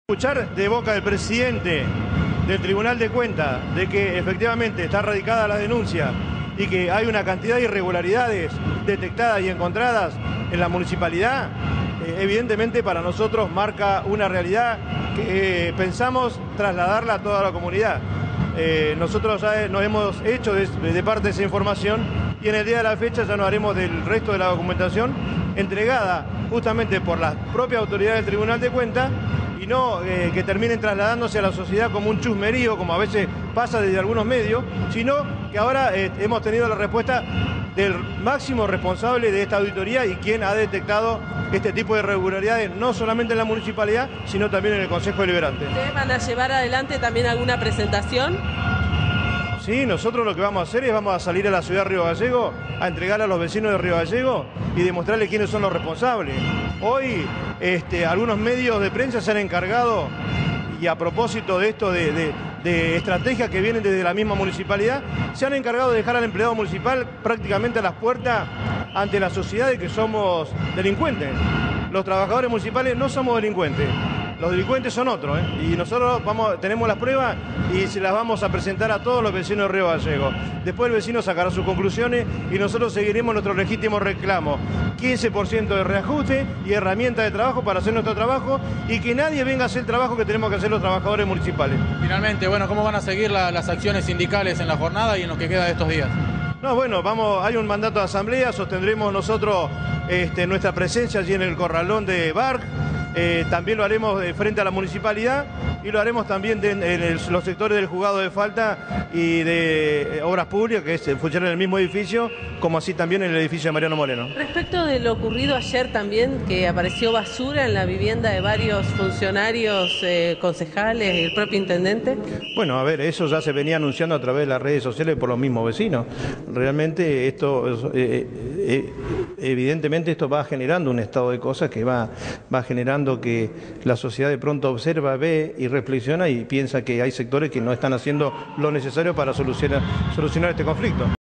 PEDRO MANSILLA- REUNIÓN CON AUTORIDADES DEL TRIBUNAL DE CUENTAS
EN OTRO TRAMO DE LA ENTREVISTA, MANSILLA SE REFIRIÓ AL RESULTADO DE LA REUNIÓN CON EL TITULAR DEL TRIBUNAL DE CUENTAS, QUIEN HABRÍA MANIFESTADO QUE “HAY IRREGULARIDADES QUE FUERON CONFIRMADAS POR EL ENTE DE CONTROL”
DESTACÓ QUE “ALGUNOS MEDIOS INSTALARON EN LA COMUNIDAD QUE LOS TRABAJADORES MUNICIPALES SOMOS DELINCUENTES Y ESTO NO ES ASÍ.”